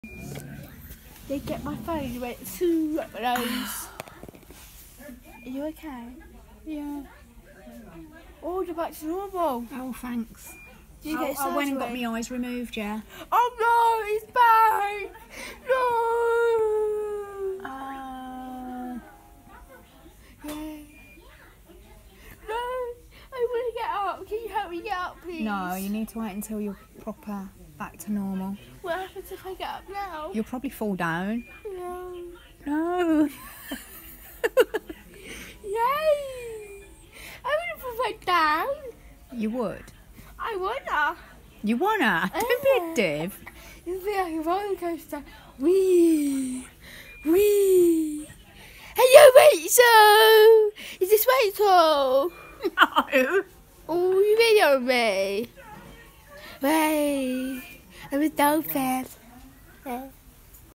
Cerys after biopsy 25th June 2019 anaesthetic high comedown
Cerys after biopsy 25th June 2019